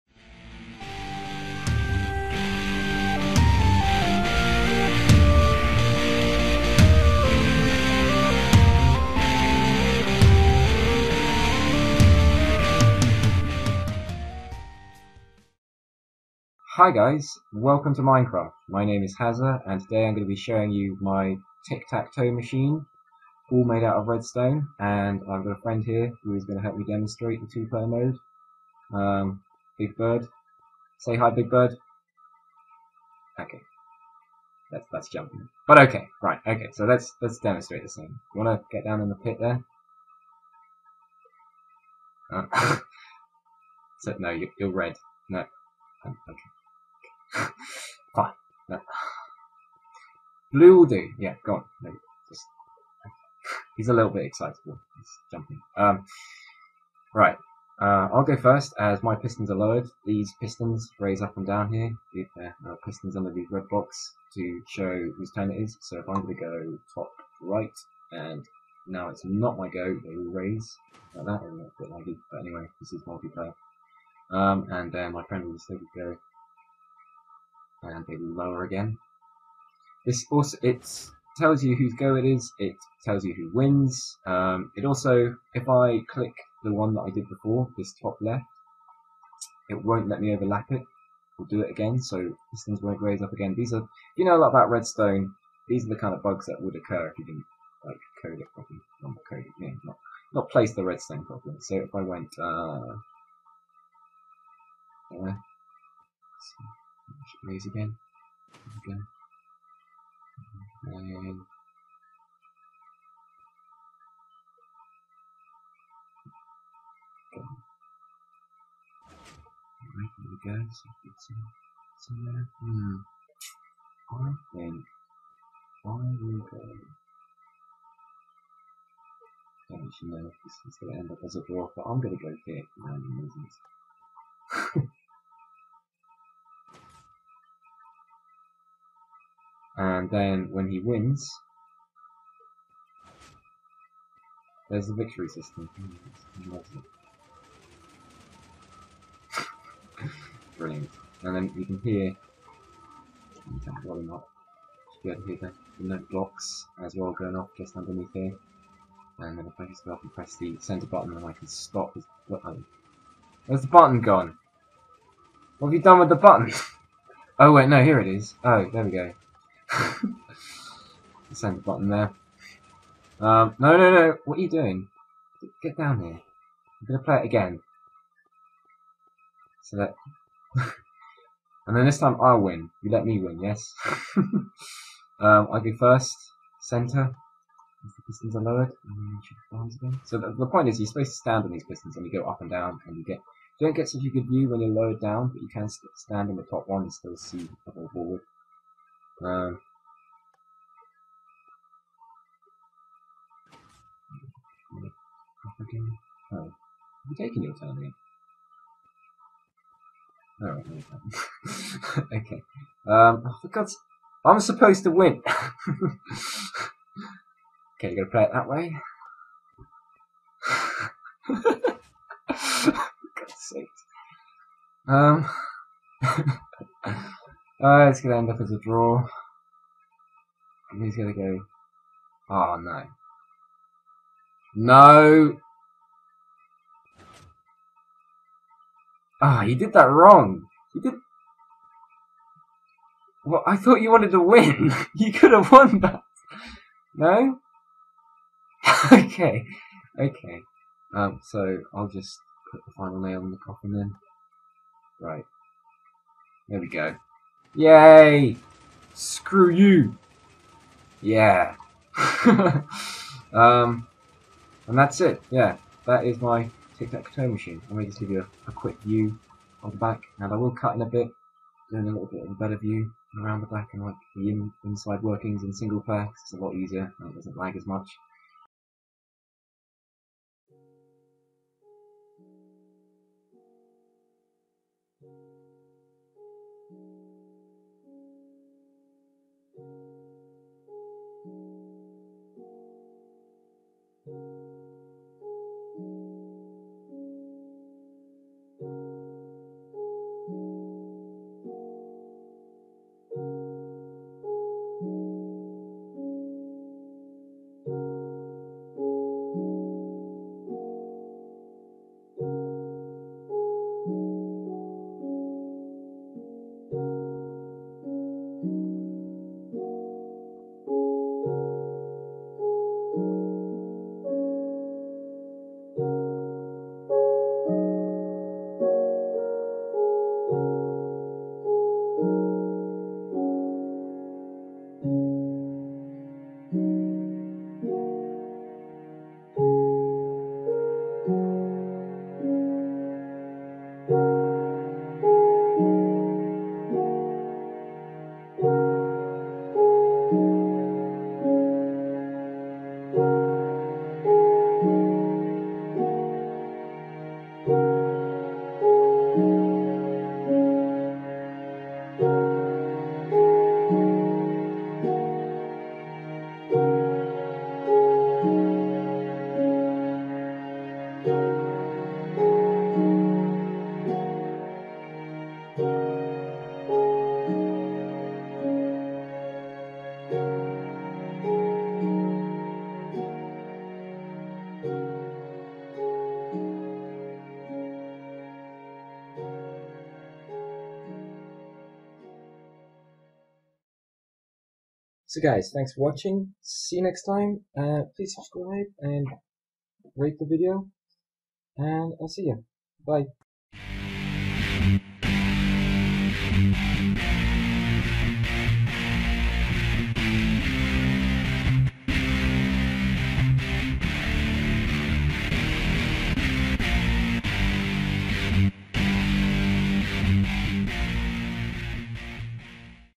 Tic Tac Toe in redstone
In this game of noughts and crosses I am planning on putting in random AI so that you can play the computer in single player.